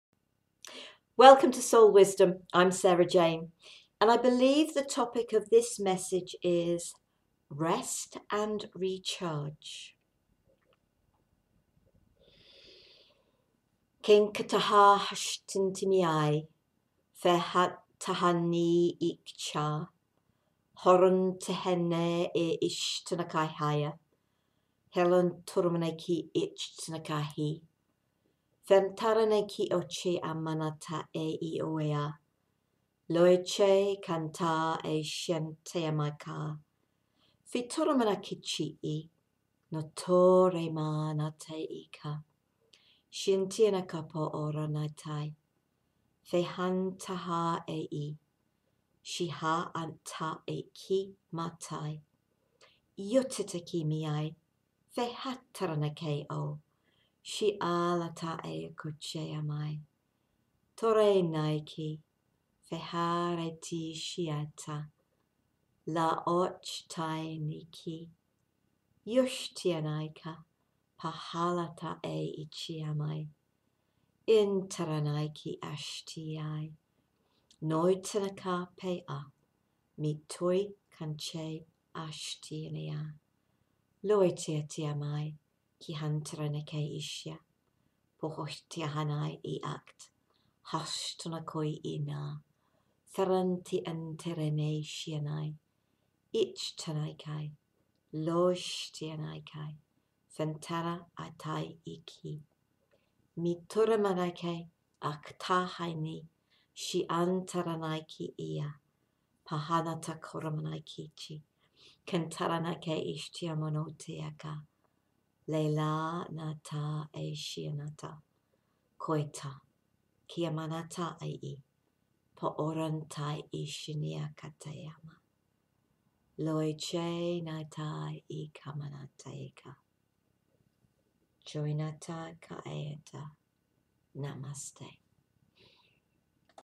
Soul Wisdom: Rest & Recharge
Sara Jane shares some Wisdom from your Soul in what she believes is our Soul Language.